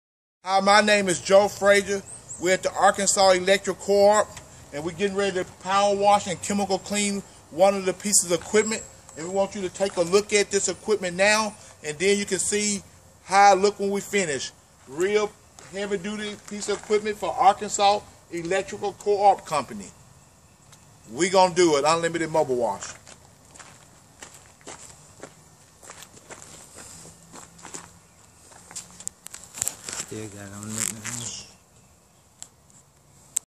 Joe Frazier of Unlimited Mobile Wash at Arkansas Electric Cooperatives, Inc.
Joe Frazier is explaining the thorough cleaning that Unlimited Wash can provide on major equipment